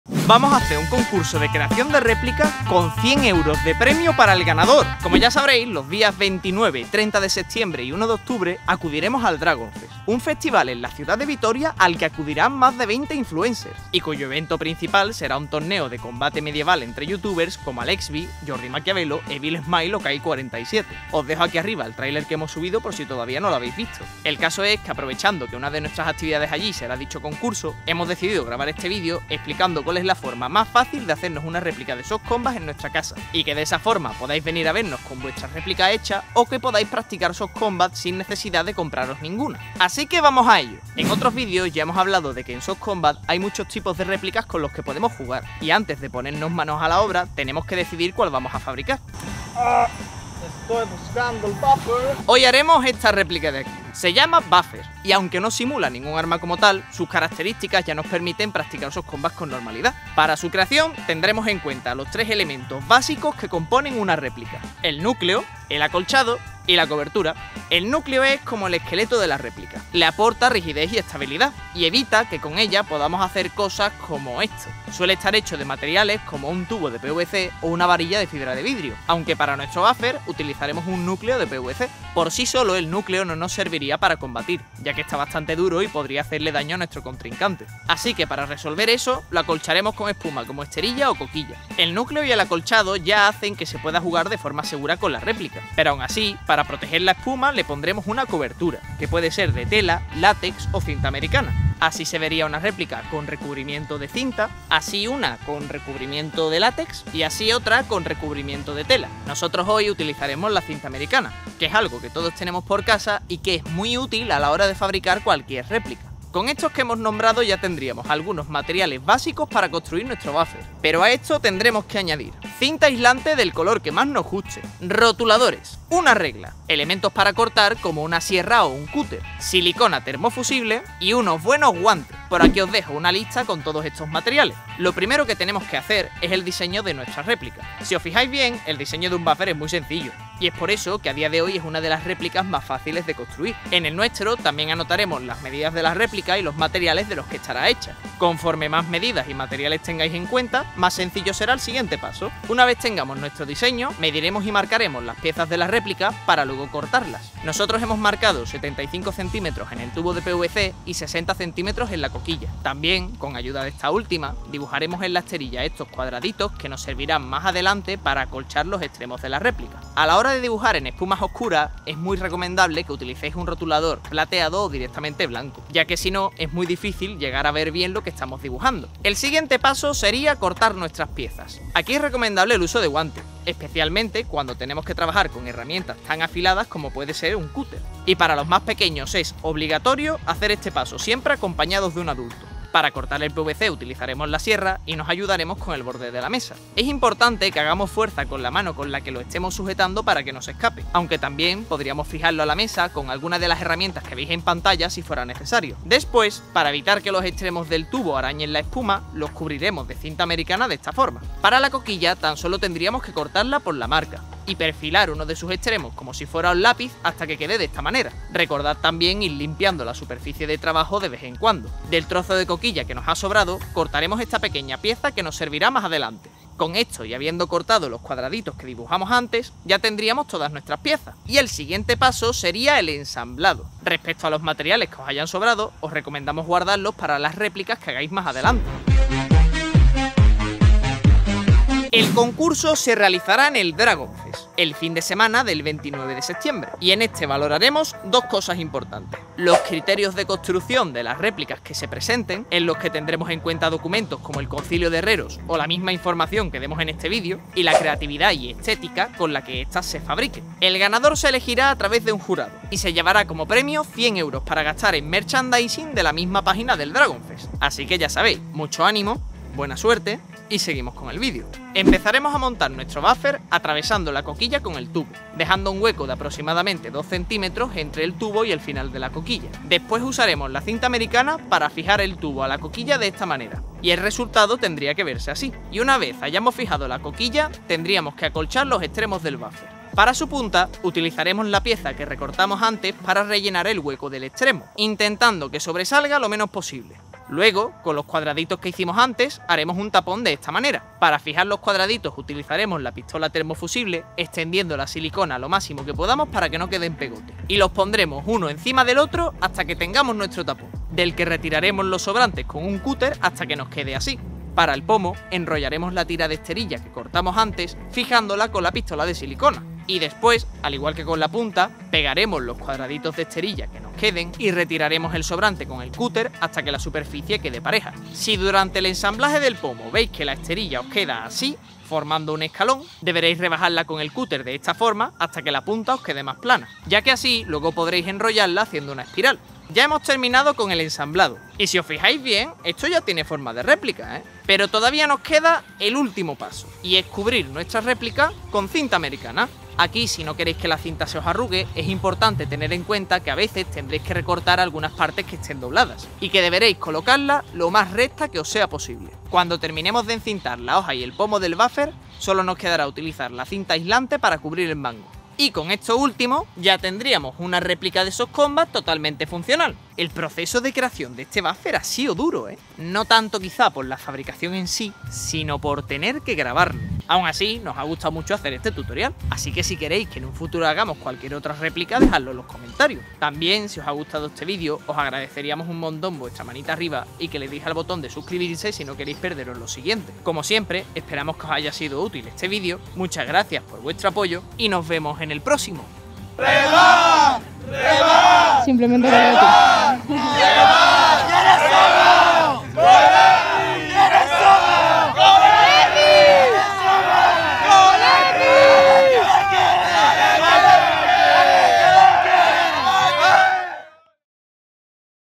¿CÓMO HACER una RÉPLICA 🛡️ de SOFTCOMBAT? - FACIL
📷 NUESTRAS REDES 📷
🟢 CAPITULOS 🟢
0:00 Introducción
0:41 El Boffer
1:00 Partes de la Réplica
1:58 Materiales
2:16 PASO 1: Diseño
2:35 PASO 2: Medir y Dibujar
3:02 PASO 3: Cortar
3:59 BASES DEL CONCURSO
4:31 PASO 4: Ensamblar
5:40 PASO 5: Encintar
6:07 ¡Suscríbete!